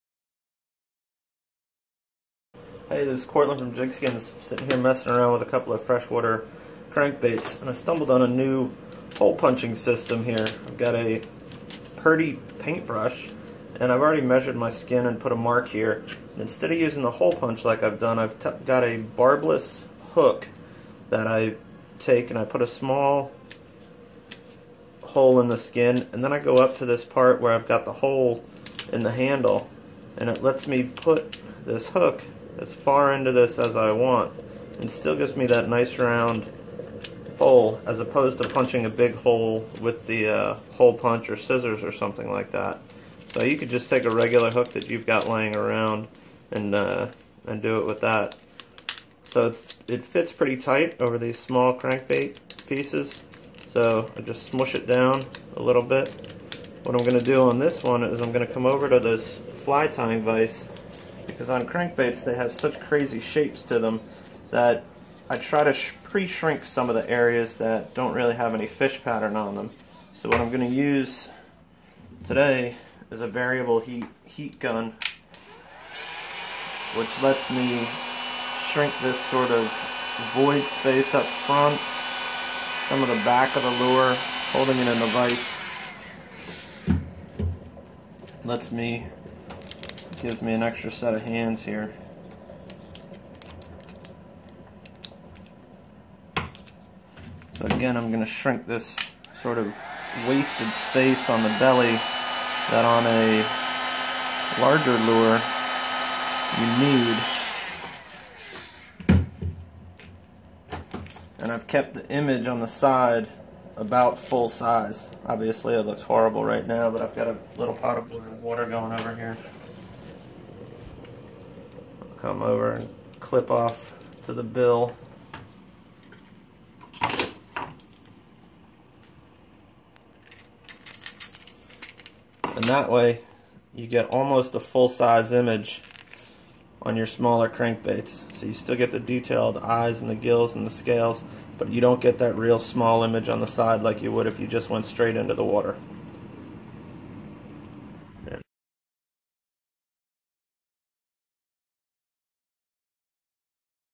CrankBait Install- JigSkinz
Some Pro-Tips on installing JigSkinz on crankbaits